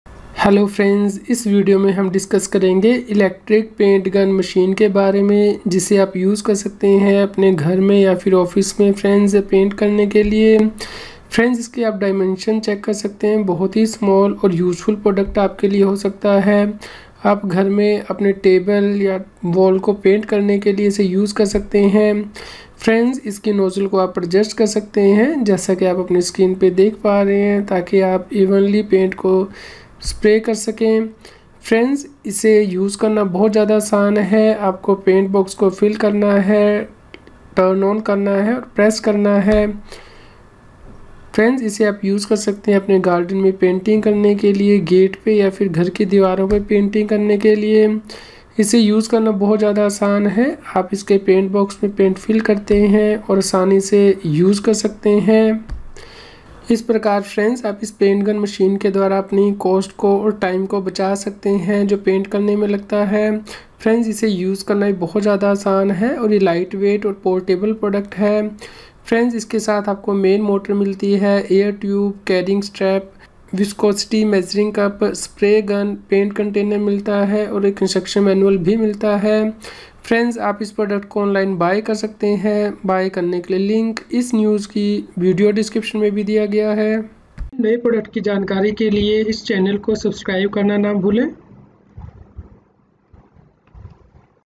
Electric Paint Sprayer Gun Machine I Portable I Ghime Paint Zoom 650 W I NewsVani
Simple one-touch operation cuts your time and paint cost in half.
very easy and convenient to use.
Portable & Light weight.
Ultra light and portable, Industrial-strength and commercial-grad.
Main Motor unit, Air tube, Carrying Strap, Viscosity measuring cup, Spray Gun, Paint Container, Multi-language Instruction Manual.